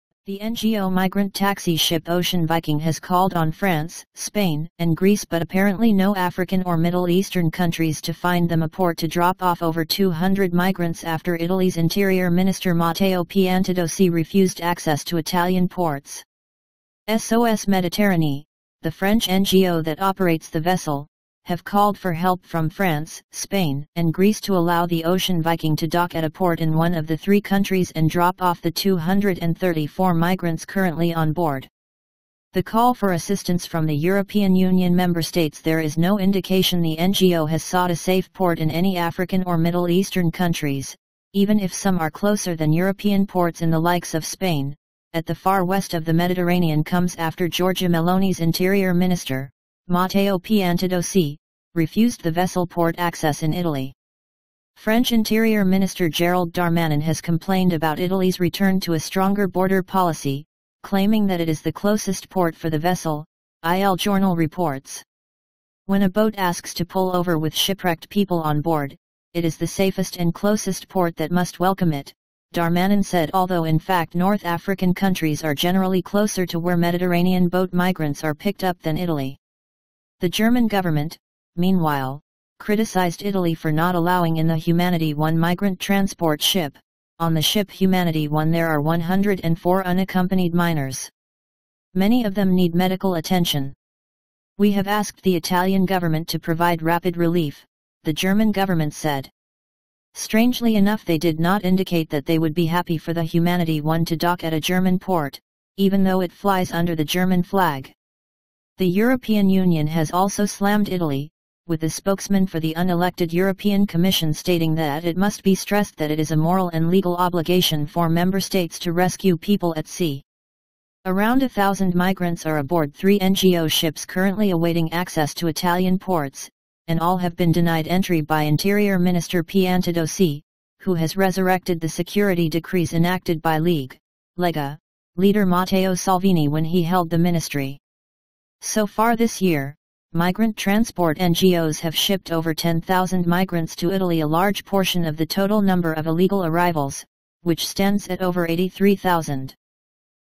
Migrant ‘taxi’ ship turns to other EU countries as Meloni’s Italy refuses entry
The NGO migrant “taxi” ship Ocean Viking has called on France, Spain, and Greece – but apparently no African or Middle Eastern countries – to find them a port to drop off over 200 migrants after Italy’s Interior Minister Matteo Piantedosi refused access to Italian ports.

SOS Méditerranée, the French NGO that operates the vessel, have called for help from France, Spain, and Greece to allow the Ocean Viking to dock at a port in one of the three countries and drop off the 234 migrants currently on board.

The call for assistance from the European Union member-states — there is no indication the NGO has sought a safe port in any African or Middle Eastern countries, even if some are closer than European ports in the likes of Spain, at the far west of the Mediterranean – comes after Giorgia Meloni’s interior minister, Matteo Piantedosi, refused the vessel port access in Italy.

French interior minister Gérald Darmanin has complained about Italy’s return to a stronger border policy, claiming that it is the closest port for the vessel, Il Giornale reports.

“When a boat asks to pull over with shipwrecked people on board, it is the safest and closest port that must welcome it,” Darmanin said — although in fact North African countries are generally closer to where Mediterranean boat migrants are picked up than Italy.

The German government, meanwhile, criticised Italy for not allowing in the Humanity 1 migrant transport ship: “On the ship Humanity 1 there are 104 unaccompanied minors. Many of them need medical attention. We have asked the Italian government to provide rapid relief,” the German government said.

Strangely enough they did not indicate that they would be happy for the Humanity 1 to dock at a German port, even though it flies under the German flag.

The European Union has also slammed Italy, with a spokesman for the unelected European Commission stating that “it must be stressed that it is a moral and legal obligation” for member-states to rescue people at sea.

Around a thousand migrants are aboard three NGO ships currently awaiting access to Italian ports, and all have been denied entry by Interior Minister Piantedosi, who has resurrected the security decrees enacted by League (Lega) leader Matteo Salvini when he held the ministry.

So far this year, migrant transport NGOs have shipped over 10,000 migrants to Italy — a large portion of the total number of illegal arrivals, which stands at over 83,000.